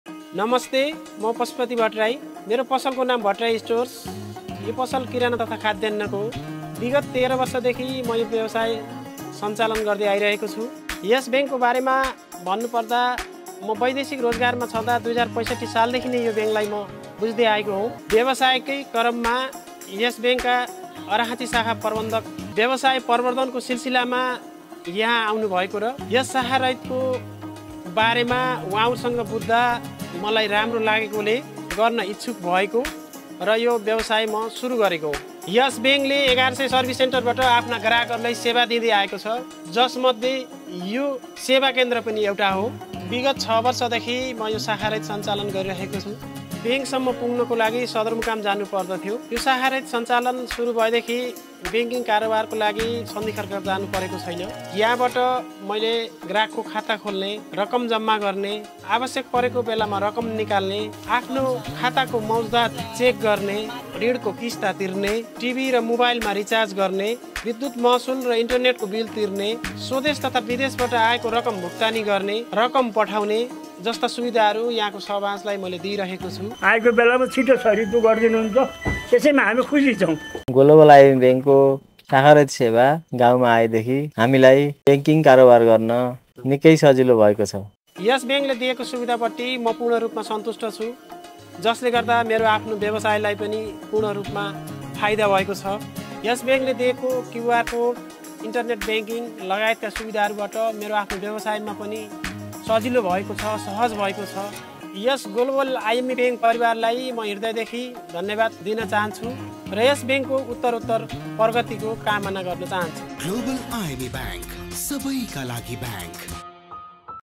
BLB banking service of Global IME Bank Ltd.
A short video of one of our BLB agent (Branchless Banking).